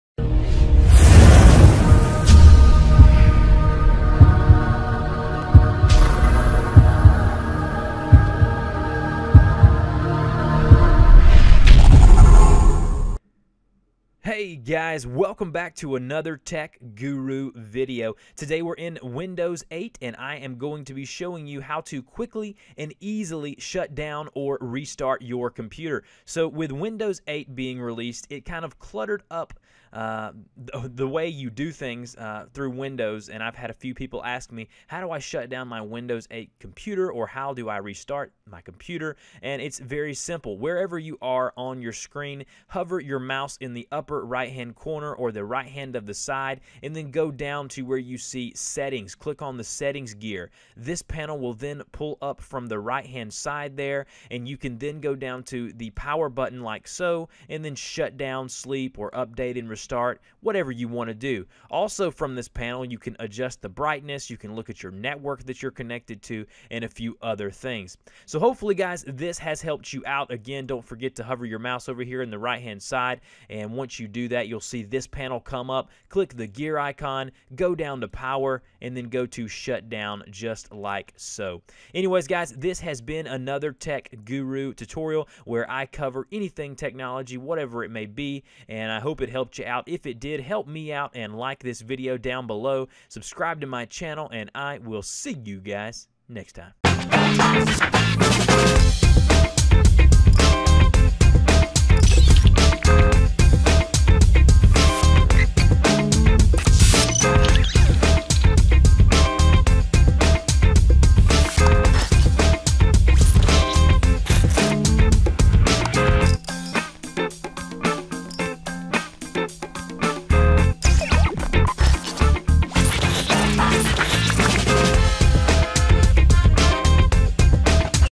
How To Shut Down or Restart Windows 8 - Windows 8 Tutorial
In this video tutorial I show you how to restart or shut down your Windows 8 computer. Although this may seem like an easy task, with the new Windows 8 configuration it can be hard to locate where to do this.

If you have any questions put them in the comment box below

dmporter74